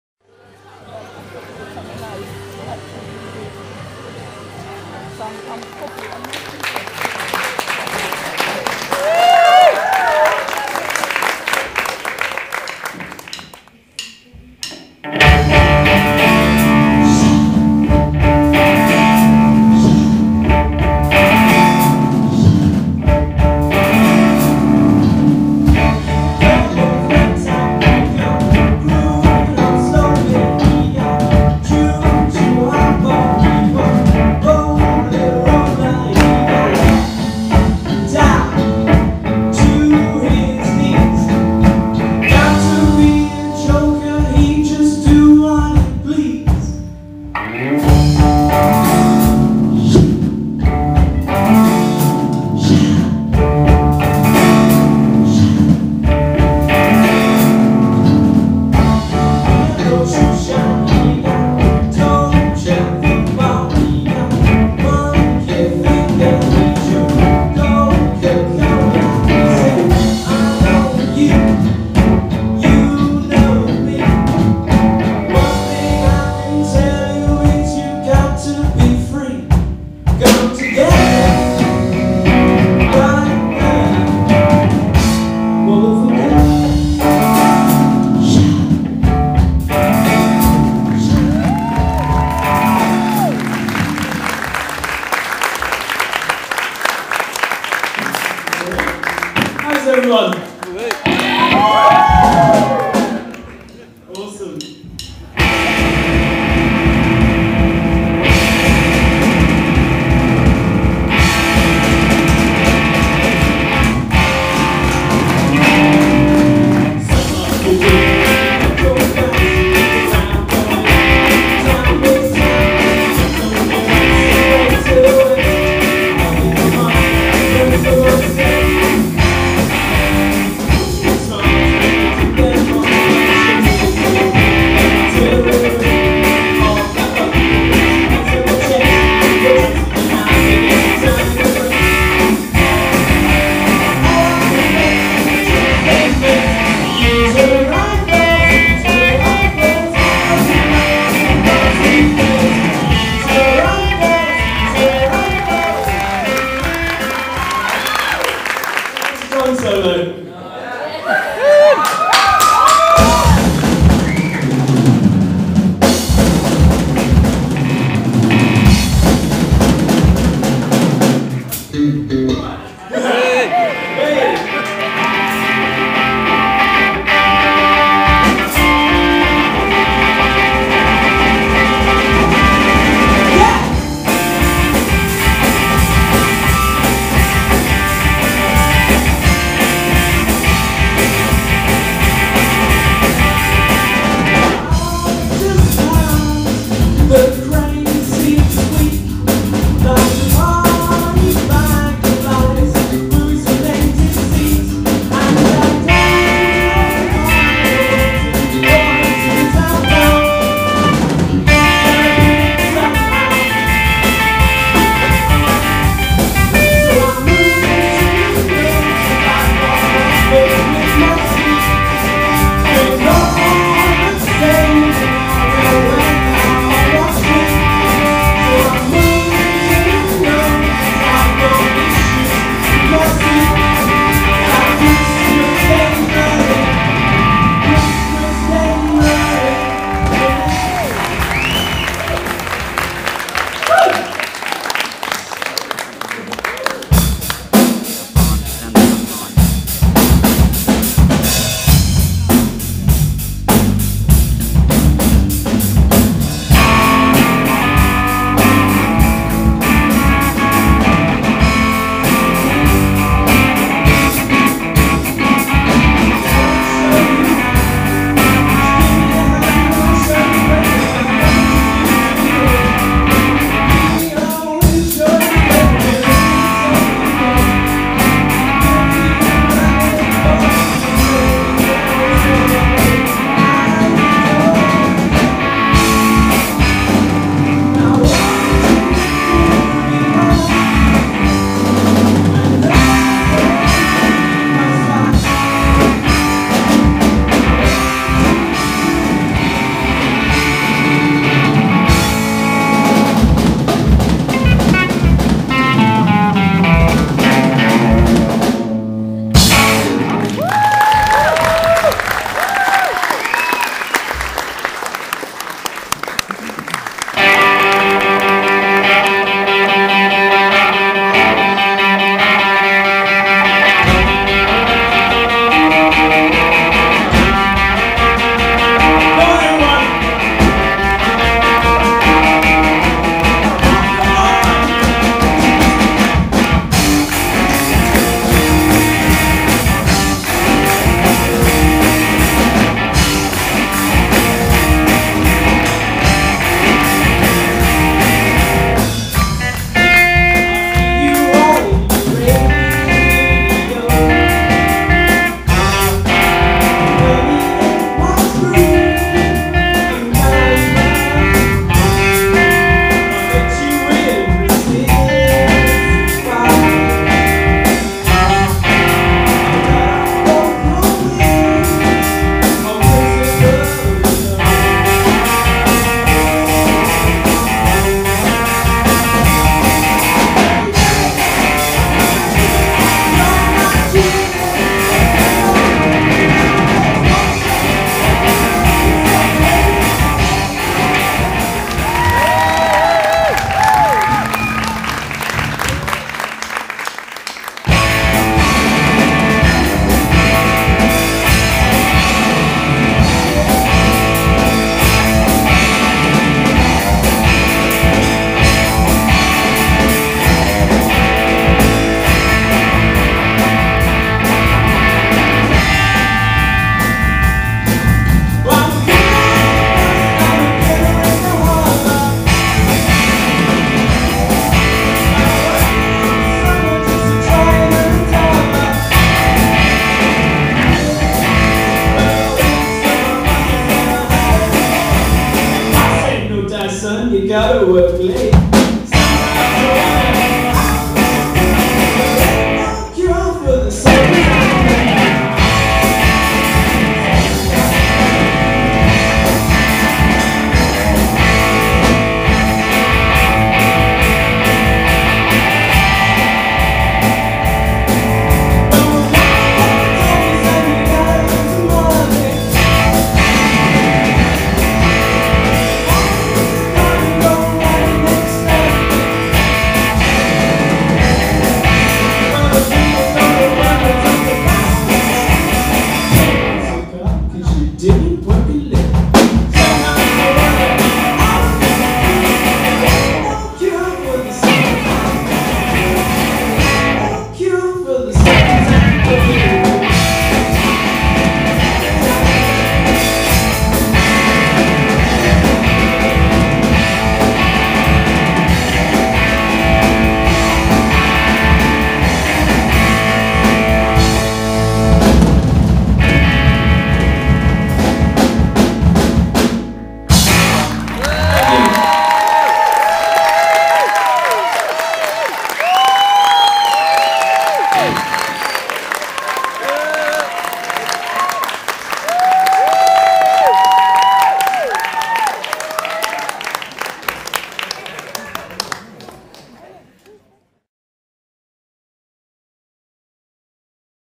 The Exceptions play Gt. Cornard Theatre
The Exceptions play Playlist One.
Great Cornard Upper Theatre
17th March 2010